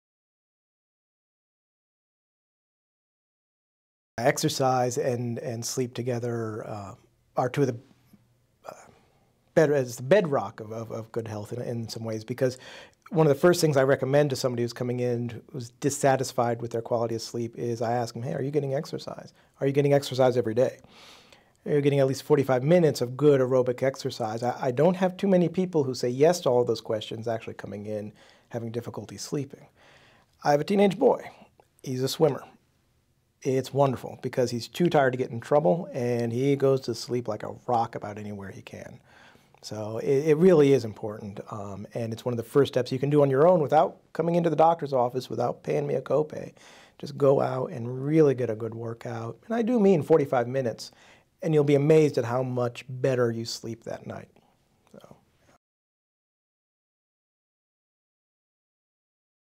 How can exercise and good sleep work together to create better health?
Exercise and sleep directly affect each other and your overall health. Exercising at the right time of day can help you get a good night' sleep, and sleeping well at night will give you the energy you need to exercise. Premier HealthNet's Dr. Christopher Lauricella talks more about how exercise and sleep work together to benefit health. You can find more answers to frequently asked questions about sleep